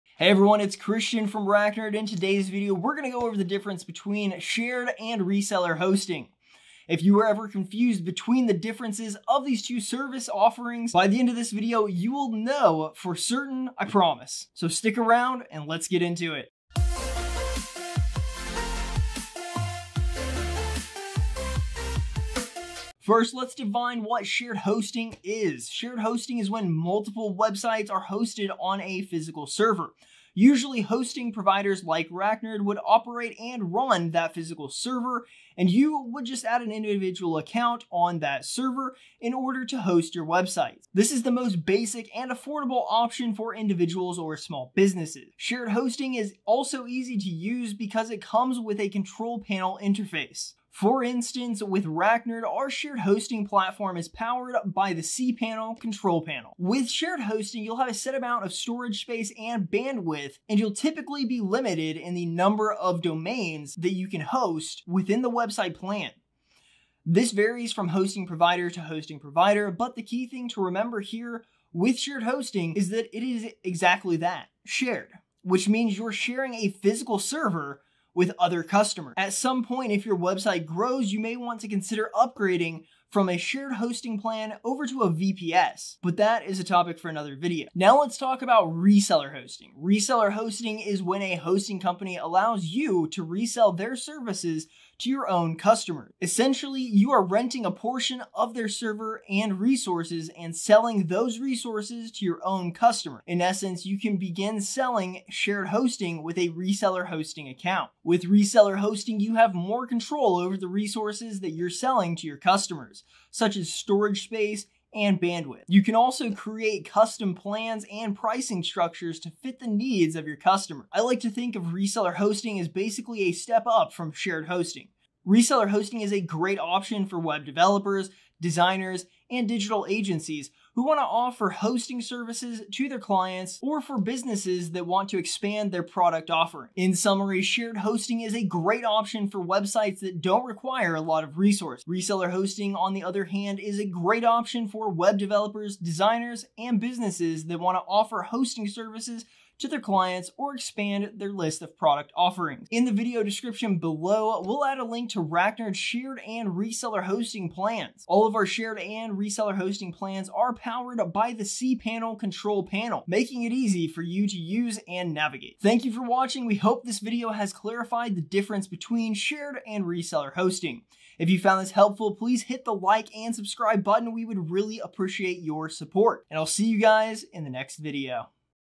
Shared Hosting vs Reseller Hosting - What's the Difference?
In this video, we'll go over the differences between: Shared Hosting vs Reseller Hosting :)